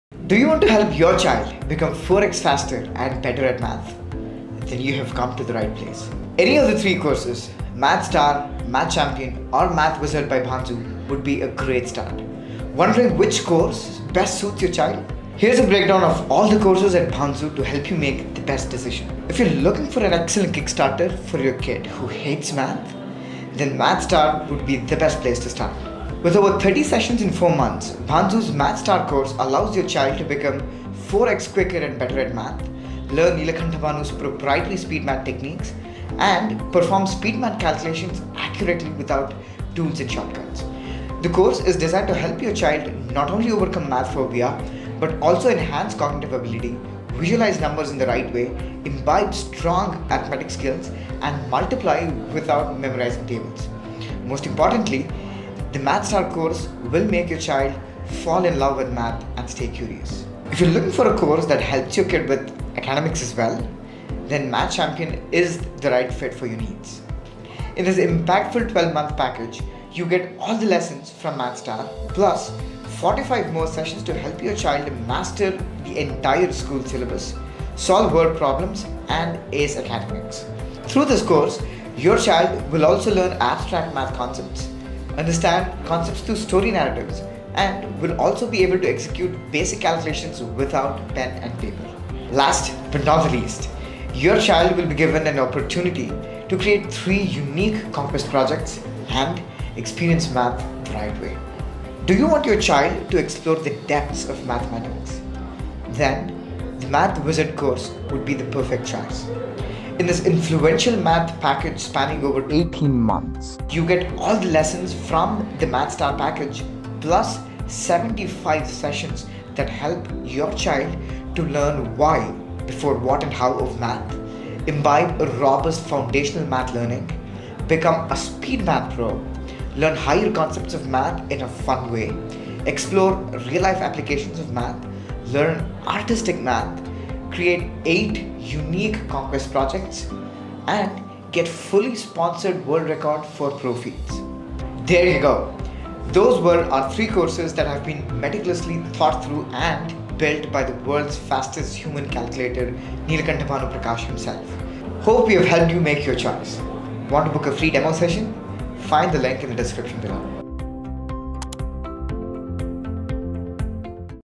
What are the different courses at Bhanzu? - Details about Bhanzu courses
In this video, Sujan Saligram, our Marketing Communications Executive, explains the difference between the various Bhanzu courses. Bhanzu Math Star, Math Champion, and Math Wizard: which course is the best for your child? Watch this video to find out.